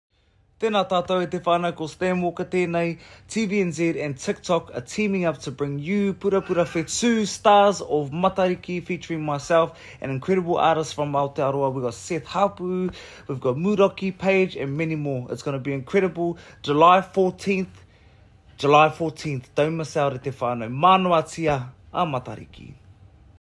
Stan Walker, Matariki 14th July 2023
Thrilled to share - 14th July myself and my full band will performing a LIVE stream from the TVNZ studios to celebrate Matariki. This is a very special time and we will be performing sons in both english and reo. There are some amazing acts I will be sharing the stage with. If you miss the LIVE stream you can catch all the of the concert here. For those over seas, Matariki marks the start of the Māori lunar calendar. Ngā mata o te ariki o .Tāwhirimātea.